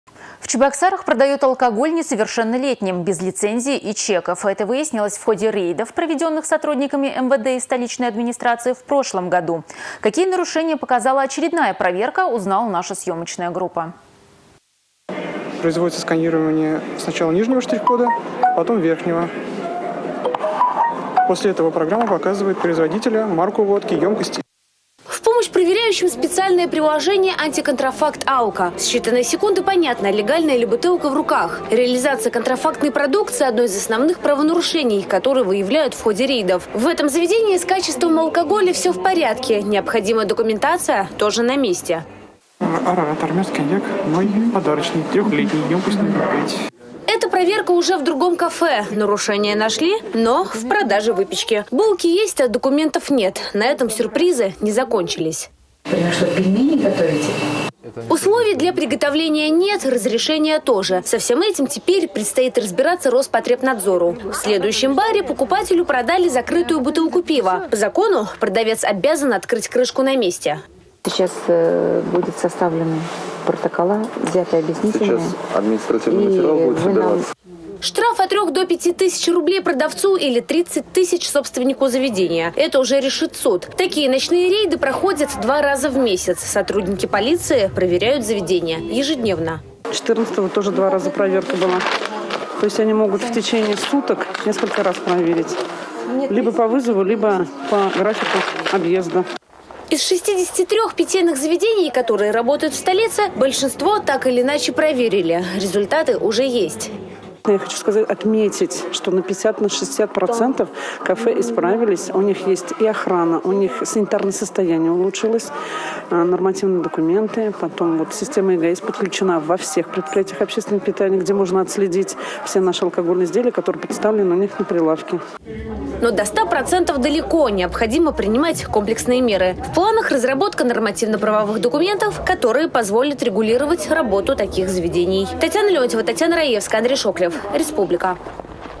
В Чебоксарах продают алкоголь несовершеннолетним, без лицензии и чеков.
Это выяснилось в ходе  рейдов, проведенных сотрудниками МВД и столичной администрации в прошлом году. Какие нарушения показала очередная проверка, узнала наша съемочная группа.